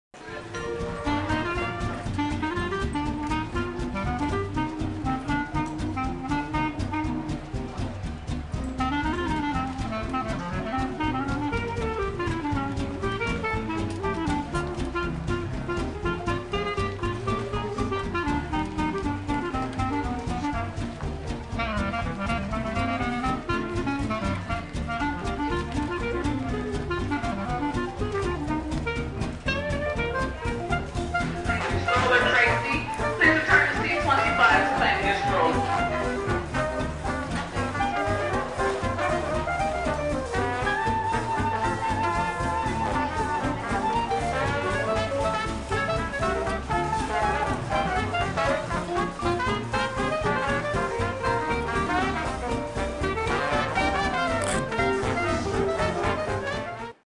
Phili Airport Rockers 3/22
Silly daily dance ditty in transit (a very full days journey from seattle to richmond virginia) inspired by all the fun things at the Philadelphia Airport (the truly magic markers, the fresh pretzel rollers and---Oh, those rocking chairs---so very cute and inviting! Thanks to total stranger for camera holding.